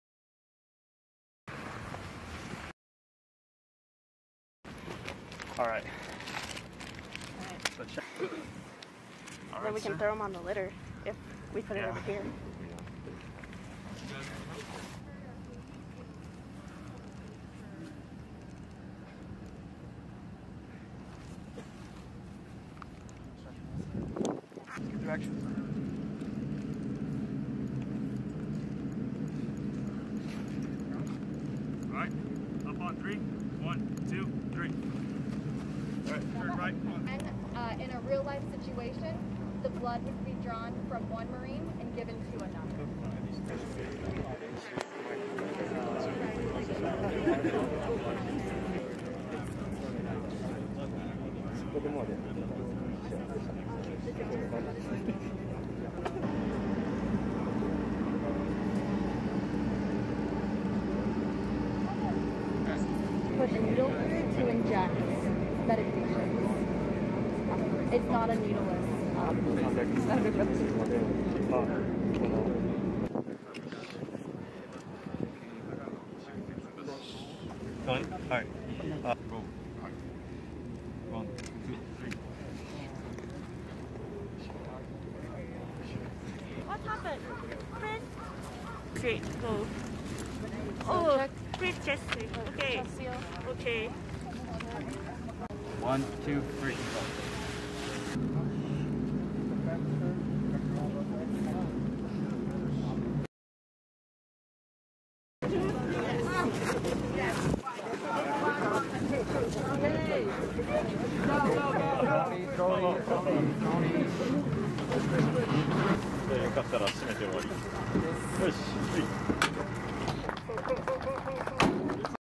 U.S. Navy Corpsmen with 3d Medical Battalion conduct bilateral Tactical
U.S. Navy Corpsmen with 3d Medical Battalion conduct bilateral Tactical Combat Casualty Care training with Japan Ground Self-Defense Force members during Keen Sword, on Amami, Japan, Nov. 10, 2022. Keen Sword is a biennial training event that exercises the combined capabilities and lethality developed between 3d Marine Division, III Marine Expeditionary Force, and the Japan Self-Defense Force. This bilateral field-training exercise between the U.S. military and JSDF strengthens interoperability and combat readiness of the U.S.-Japan Alliance.(U.S. Marine Corps photo by Sgt. Levi J. Guerra)

Keen Sword 23: U.S Navy Corpsmen and JGSDF members conduct bilateral Tactical Combat Casualty Care training
JAPAN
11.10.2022
Video by Sgt. Levi Guerra
3rd Marine Division